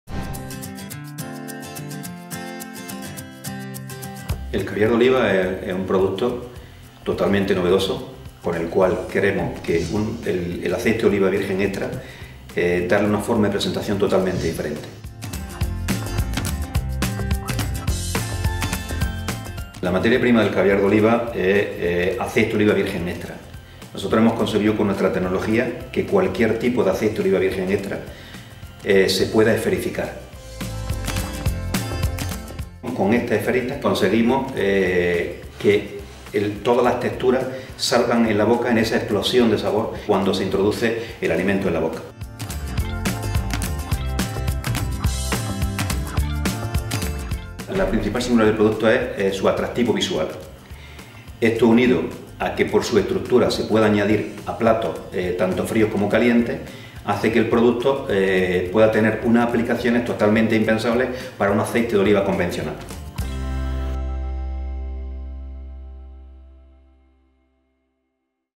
Caviar de Aceite de Oliva. Castillo de Locubín. Jaén
La vocación del Grupo Elayo, ubicado en Castillo de Locubín (Jaén), es impulsar la innovación en el sector del olivar con una visión distinta a la tradicional, de modo que desde el grupo se están desarrollando nuevas tecnologías que permiten aprovechar las distintas partes de la aceituna y la hoja del olivo sin la generación de residuos y con métodos respetuosos con el medio ambiente con el objetivo de poner a disposición de los mercados una nueva gama de productos sanos y naturales.
El Gobierno ha concedido al Grupo Elayo dos préstamos por más de un millón de euros para continuar impulsando la innovación en el sector del olivar.

Polígono Industrial El Cerezo. C/ Almendro, 31, C.P. 23670